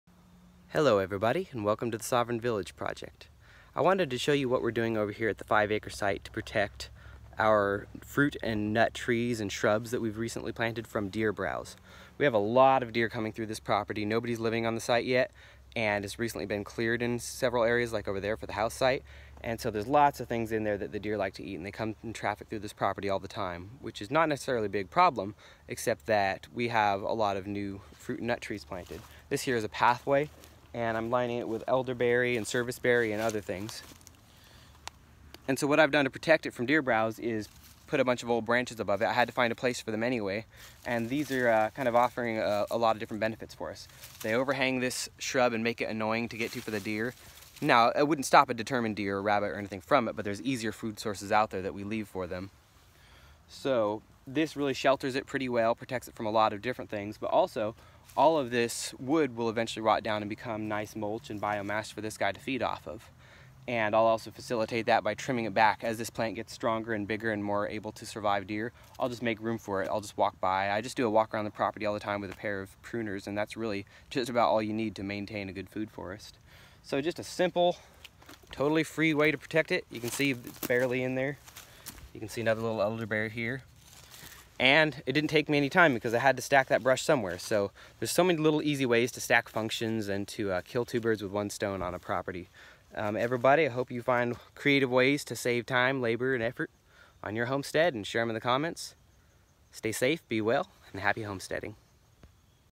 Protect Your Seedlings From Deer - Free & Easy!
A simple way to protect new fruit and nut plantings from deer and other wildlife.